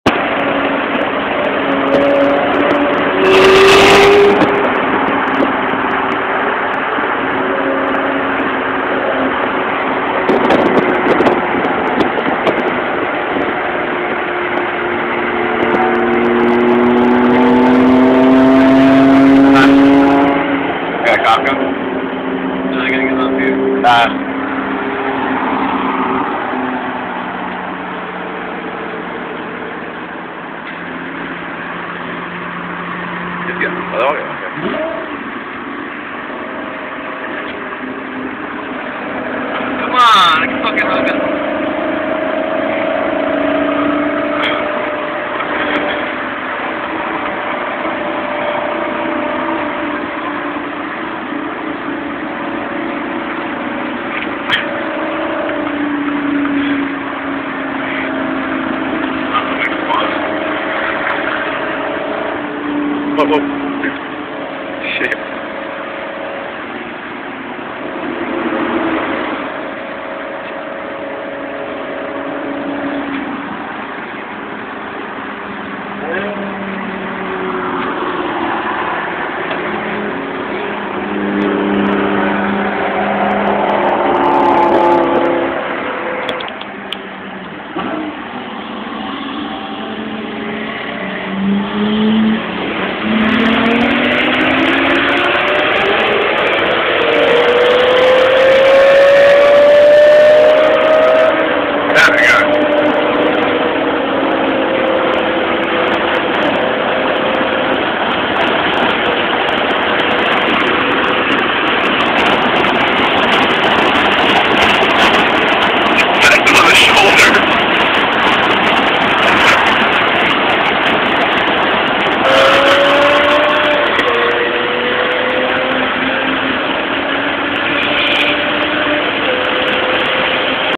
Lambo, BMW M5 Highway run
Exotics Rally meet.  Lamborghini Gallardo and E60 BMW M5 on the highway.